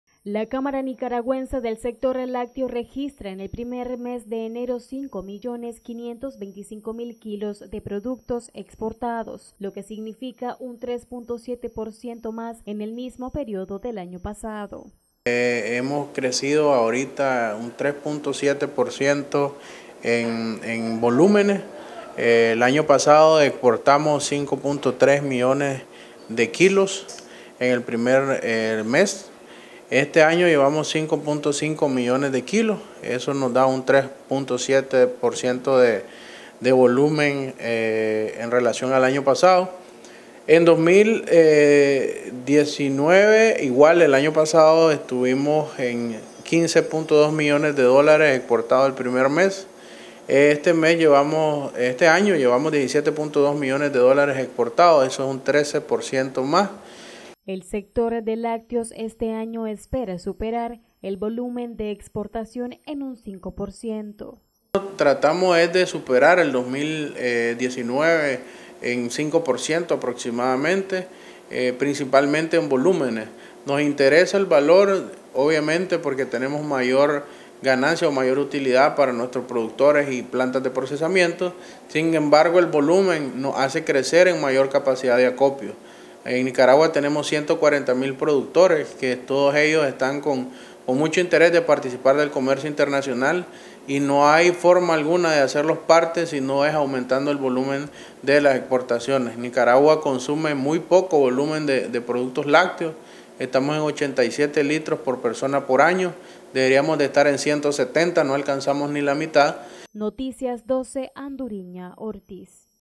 Registran en enero incremento de 3.7% de exportación de productos lácteos
CANISLAC registra en el mes de enero 5 millones 525 mil kilos  de productos  exportados, lo que significa un 3.7%  más en el mismo periodo del año pasado.